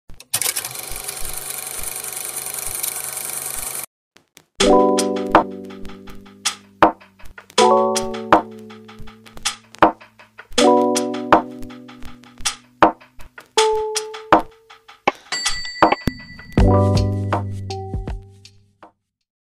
叮~请查收你的边牧小可爱—border collie Mia
突然翻出来一个米娅小时候的照片，看完萌化我了